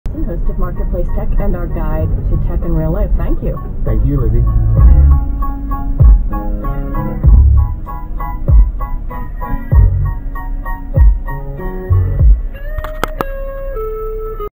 Passing Stopped School Bus 08/02/15
Guy passes a stopped school bus with arm out.  Between Fuquay-Varina and Holly Springs